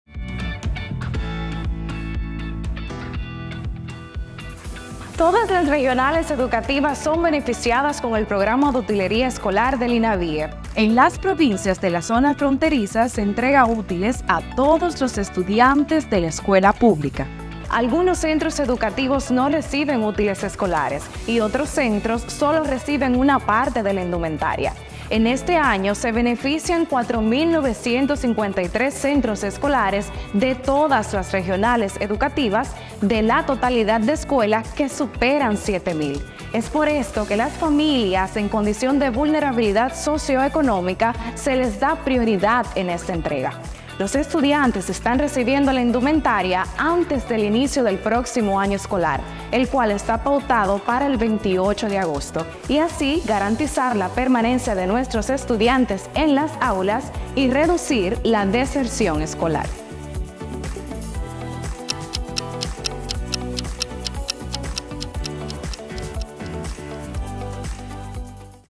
¡La utilería escolar beneficia a los estudiantes! 🙌🤓
Todas las regionales educativas son impactadas con la UtileríaATiempo 📚 algunas de forma parcial y otras al 100% de la matrícula escolar de los centros beneficiados, como es el caso de las provincias de la zona fronteriza 📍.

Este año estamos entregando la totalidad de 650 mil kits escolares distribuidos en los 4,953 centros seleccionados de todas las regionales y distritos educativos 🇩🇴✏️.

La utilería que entregamos es totalmente gratis 🙌y está totalmente prohibido su comercialización 🚫.

Los padres, madres y/o tutores de los estudiantes beneficiados no tienen que destinar recursos de sus presupuestos familiares para la compra de la indumentaria que usarán para recibir el pan de la enseñanza 👧☺️.

Con esta acción de entregar la UtileríaATiempo 📖, estamos beneficiando económicamente a las familias y contribuyendo a reducir los riesgos de deserción escolar al tiempo que mejoramos la educación dominicana .

Y como ya saben, para el año escolar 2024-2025 ningún escolar quedará sin su utilería 📒. ¡Todos serán beneficiado gracias a la entrega universal de la indumentaria escolar! 💙👞👕🧦👖.

INABIE: Beneficiando al presente y futuro.